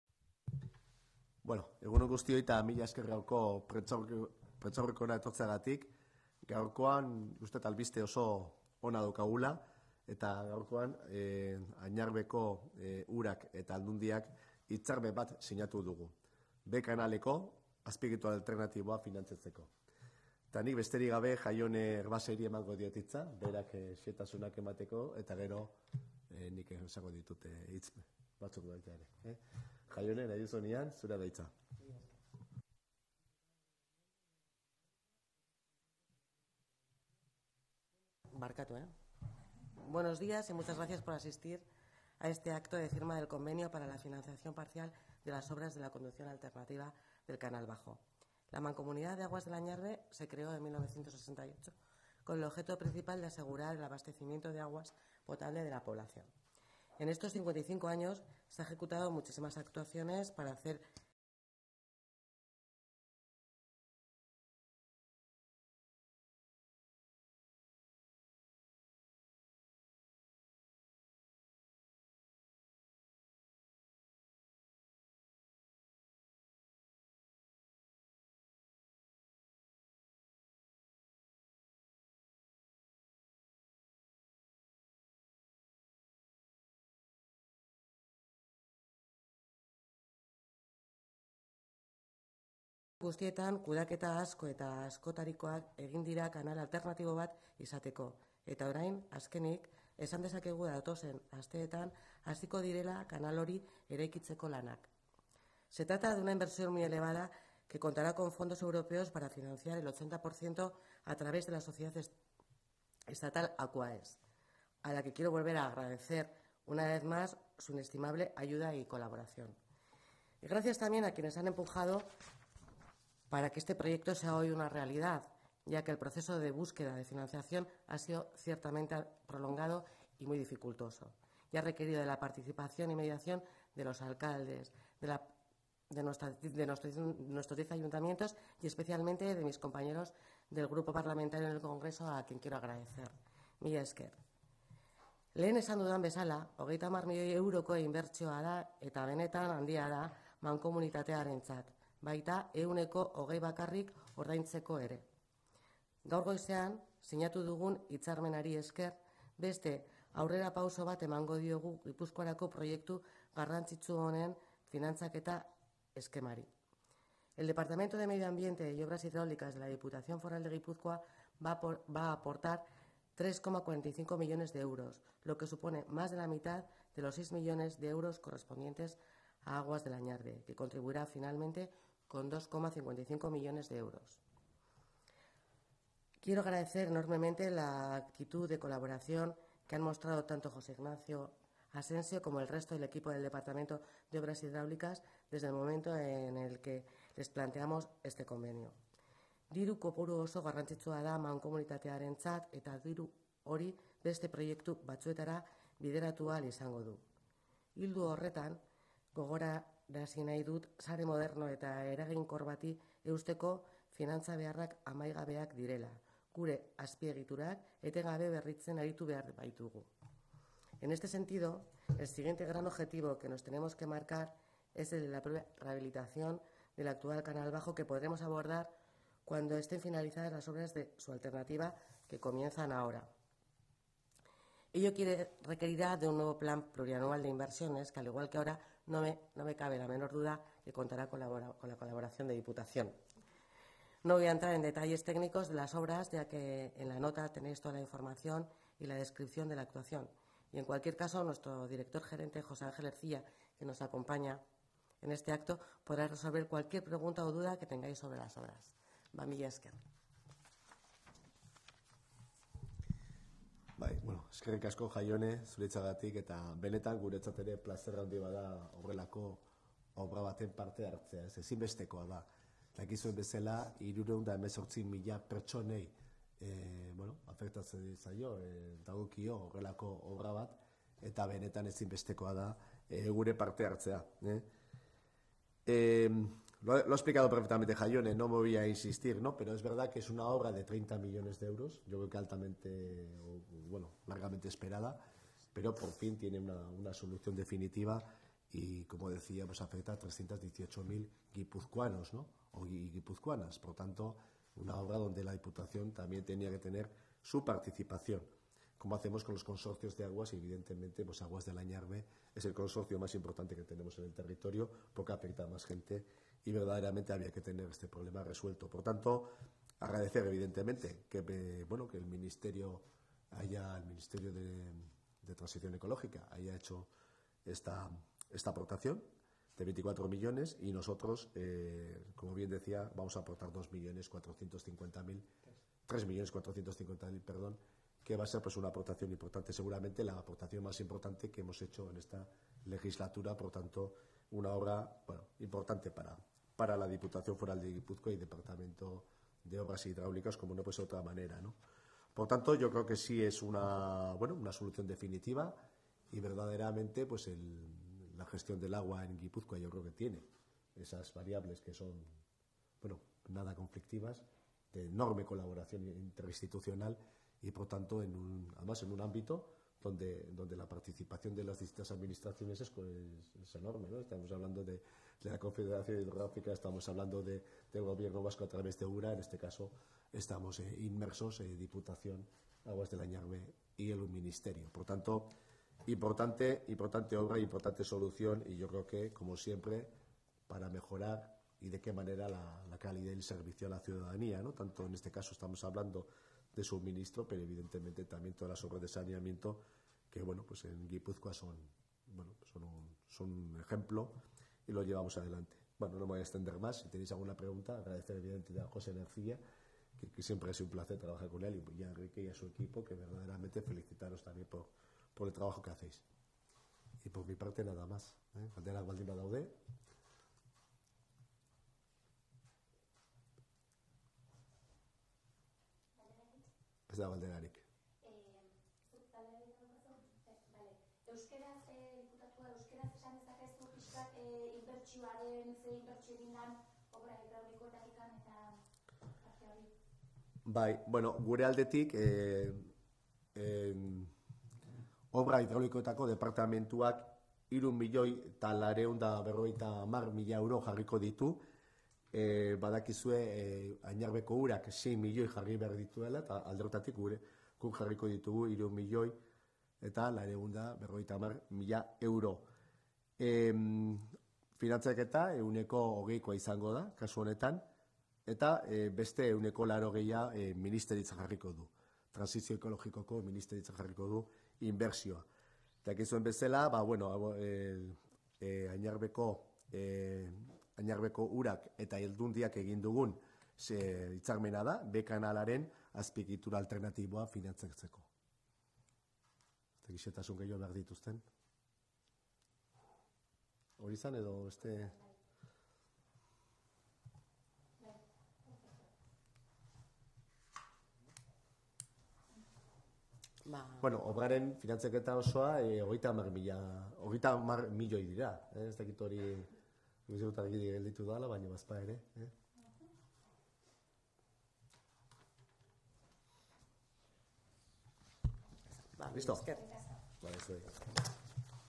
Donostialdearako Behe Kanalaren azpiegitura / Infraestructura del Canal Bajo para Donostialdea
Foru Aldundiko Ingurumen Departamentuak eta Añarbek hitzarmen bat sinatu dute Behe Kanalaren ordezko eroanbidea finantzatzeko. /
El Departamento de Medio Ambiente de la Diputación y Añarbe han suscrito un convenio para la financiación de la conducción alternativa del Canal Bajo.